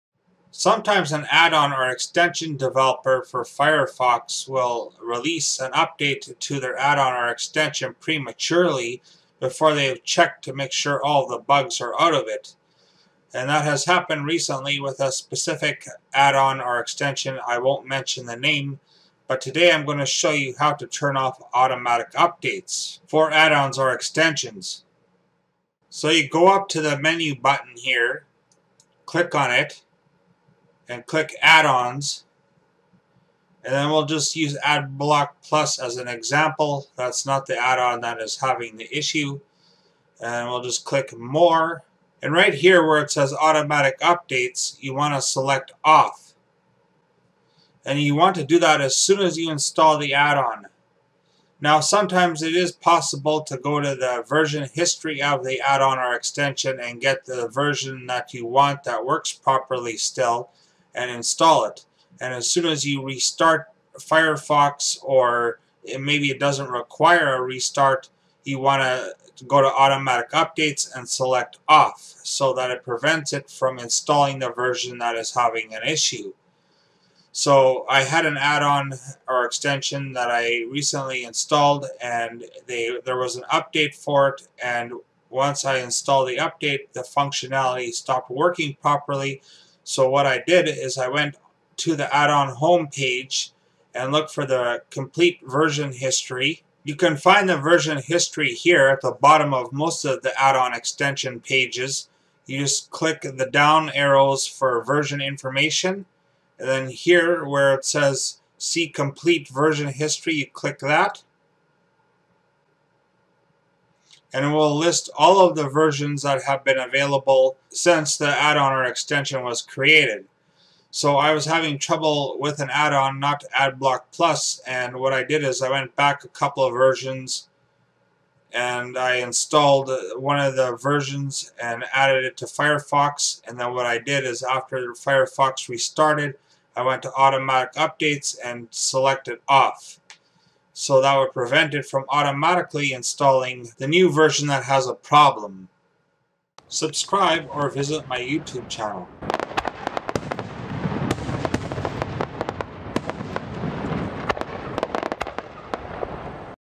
Disable Automatic Updates for Firefox Add-ons
Sometimes add-ons/extensions do not function as intended. So you may need to use a previous version. This video shows you how to disable automatic updates for add-ons/extensions in Firefox.